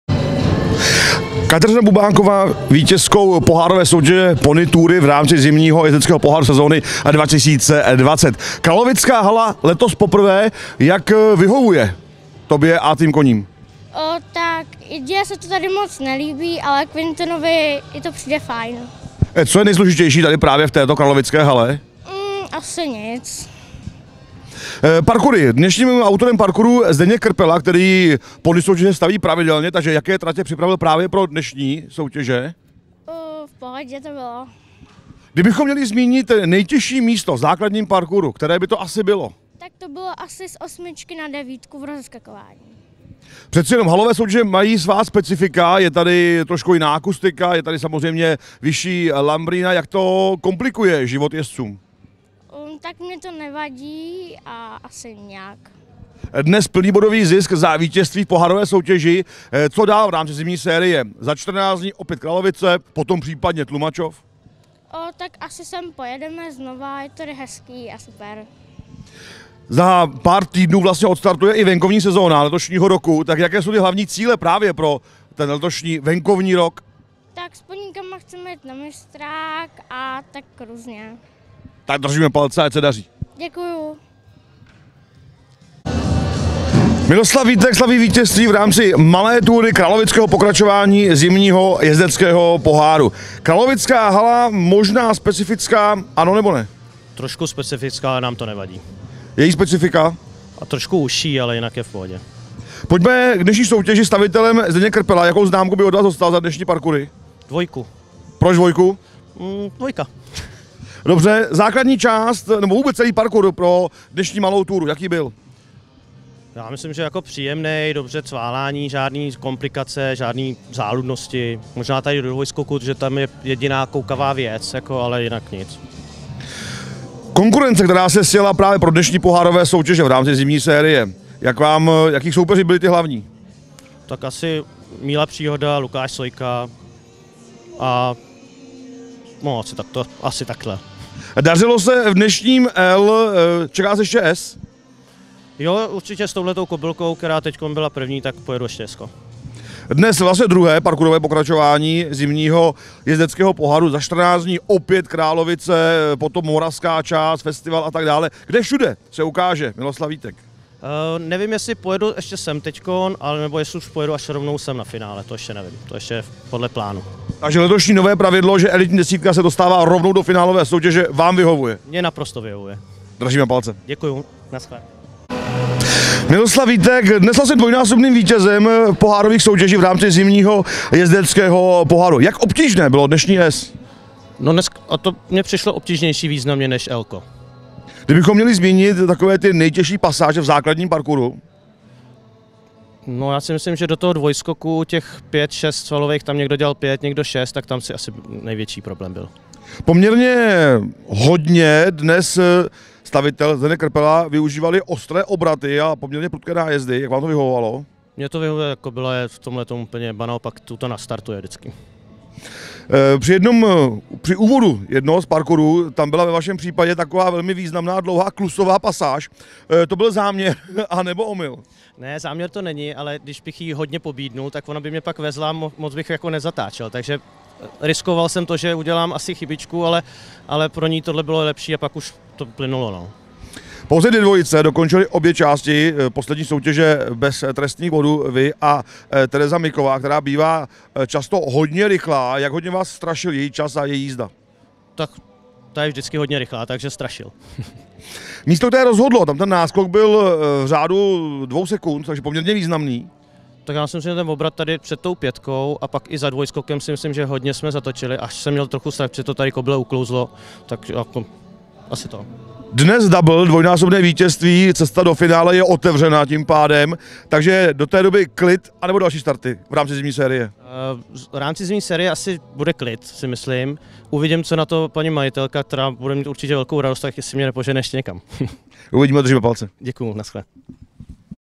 Tři soutěže, dva vítězové. Bubáková a Vítek hodnotí královický pohár
Druhý parkurový Zimní jezdecký pohár ČJF v Královicích u Slaného měl ve třech seriálových soutěžích pouze dva vítěze. Jak jsou spokojeni se svými výkony Kateřina Bubáková a Miloslav Vítek a jak hodnotí náročnost soutěží, to prozradili pro EquiTV.cz.